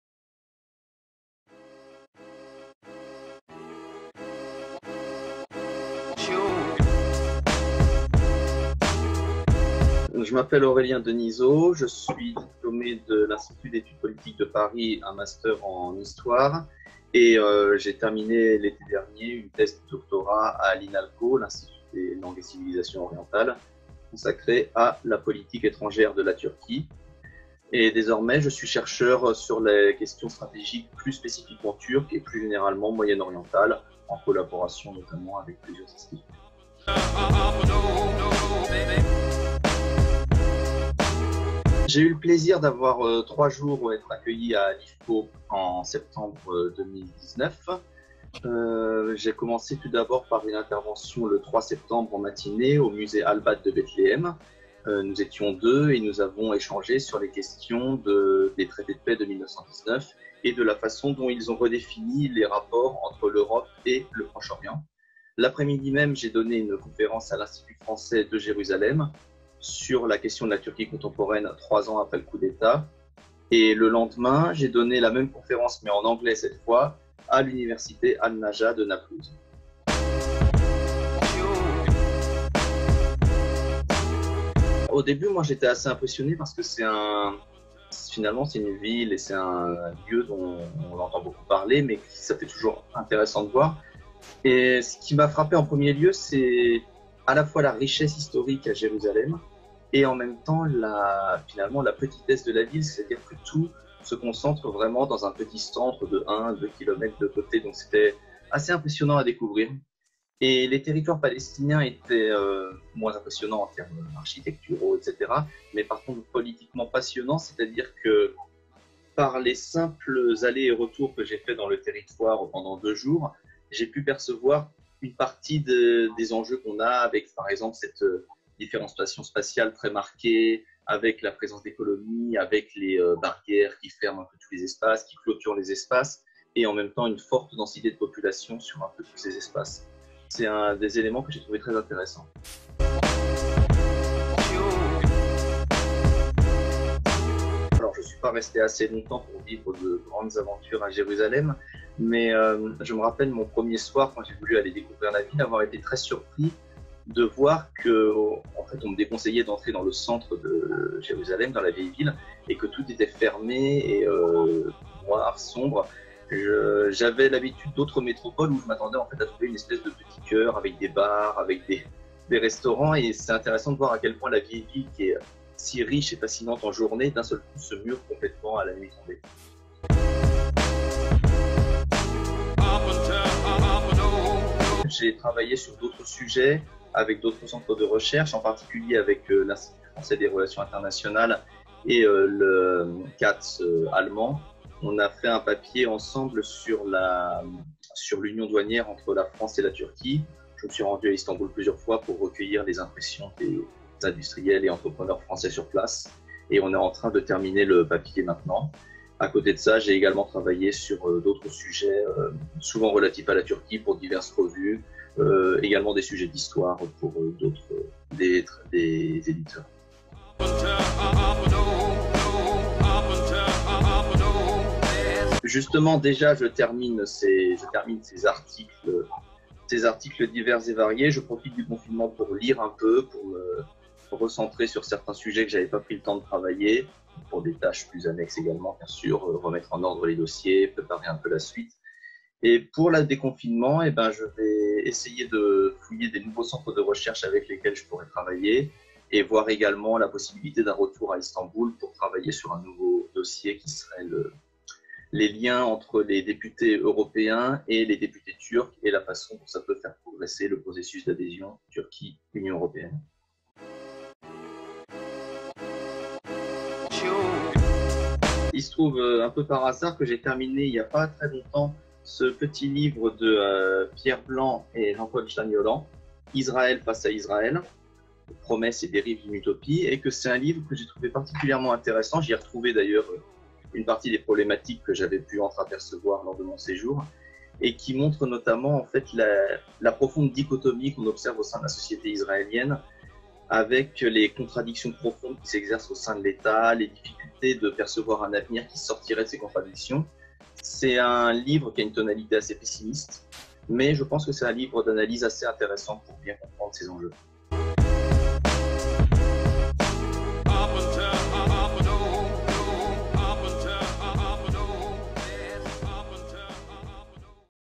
تذكار من المعهد الفرنسي في القدس أورليان دينيزو Souvenir de l’IFJ : Aurélien Denizeau
تذكار من المعهد الفرنسي في القدس أورليان دينيزو

شارك المعهد الفرنسي للقدس والمعهد الفرنسي للشرق الأدنى في تنظيم سلسلة من المؤتمرات في عام 2019 بدعم من المعهد الفرنسي في باريس (Fonds d'Alembert) حول موضوع العلاقات بين أوروبا والشرق الأوسط لمدة قرن تحت العنوان التالي: "1919/1989/2019 / نظرات متقاطعة- أوروبا – الشرق الأوسط". في المؤتمر الأول في بيت لحم، كان من دواعي سرورنا استقبال أورليان دينيزو، الباحث في المعهد الوطني للغات والحضارات الشرقية (INALCO) في باريس. مع أبهر السقا، أستاذ علم الاجتماع في جامعة بيرزيت، تمكن من الحديث عن تأثير عام 1919 في الشرق الأوسط لمدة قرن. كما قدم محاضرتين حول "السياسة التركية المعاصرة بعد ثلاث سنوات من الانقلاب العسكري" في القدس ونابلس. وهو يخبرنا عن ذلك :

Souvenir de l’Institut français de Jérusalem : Aurélien Denizeau

L’Institut français de Jérusalem (IFJ) et L’IFPO (Institut français du Proche-Orient) ont co-organisé en 2019 un cycle de conférences soutenu par l’Institut français de Paris (Fonds d’Alembert) autour de la thématique des relations entre l’Europe et le Proche-Orient depuis un siècle, sous le titre suivant : “1919/1989/2019 / Regards croisés Europe – Proche-Orient”. Pour la première conférence à Bethléem, nous avons eu le plaisir d’accueillir Aurélien Denizeau, chercheur à l’Institut National des Langues et Civilisations Orientales (INALCO) à Paris. Avec Abaher El Sakka, professeur de sociologie à l’Université de Birzeit, il a pu réfléchir sur l’impact de l’année 1919 au Proche-Orient depuis un siècle. Il a également proposé deux conférences sur “La politique Turque contemporaine trois ans après le coup d’Etat militaire” à Jérusalem et Naplouse. Il nous raconte